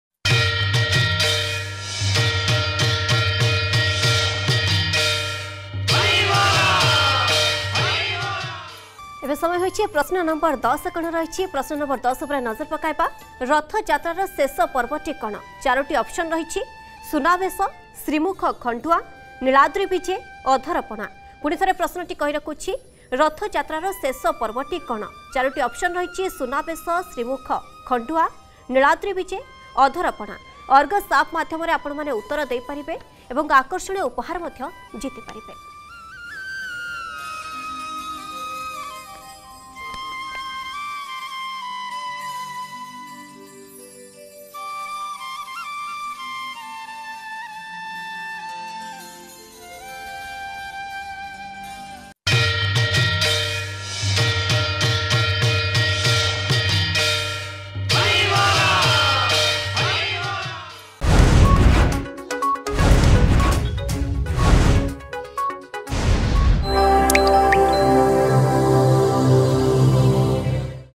ଅର୍ଗସ ନିଳାଦ୍ରୀବିଜେ କୁଇଜ୍ ପ୍ରତିଯୋଗିତା...
10-ରଥଯାତ୍ରାର ଶେଷ ପର୍ବଟି  କଣ ?
ପ୍ରଶ୍ନର ଉତ୍ତର ଦେବା ପାଇଁ ଡାଉନଲୋଡ କରନ୍ତୁ ଆମ ଅର୍ଗସ ଆପ୍